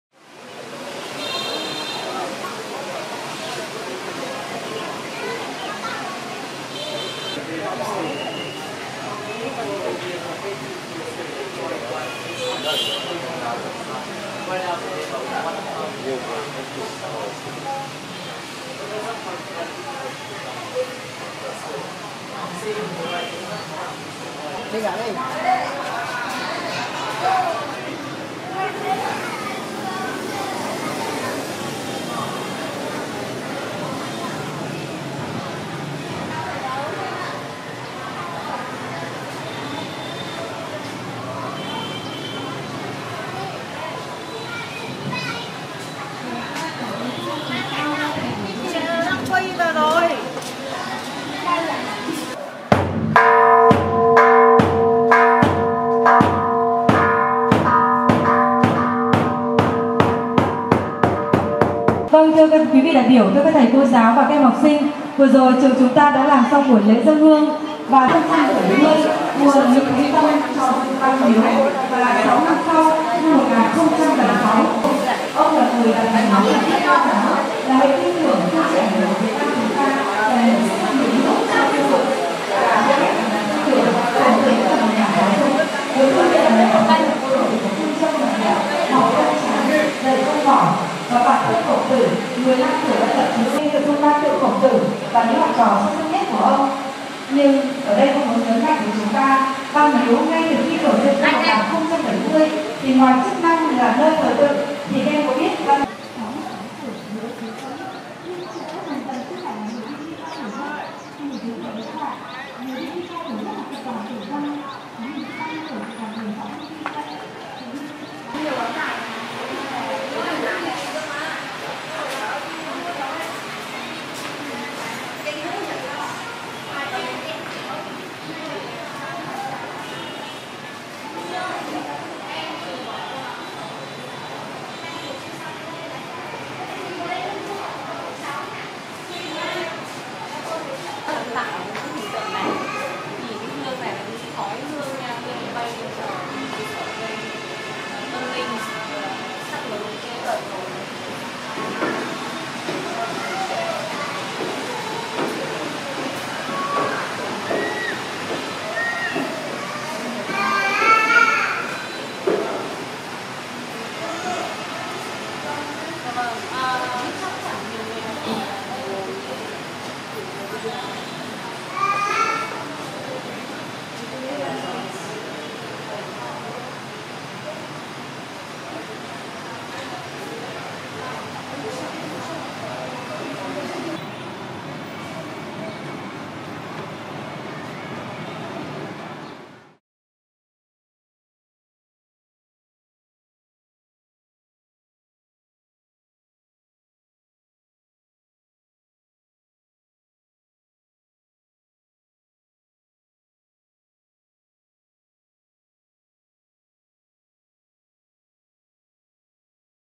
The Temple of Literature During Graduation Season in Hanoi
You won't find the Temple of Literature more crowded than during the school graduation season. One school class after the other received their blessings and congratulations.

We especially loved the turtle columns with names and birthplaces engraved of all the students who passed the royal exams.

Read all about the Temple of Literature on our Hanoi Travel Blog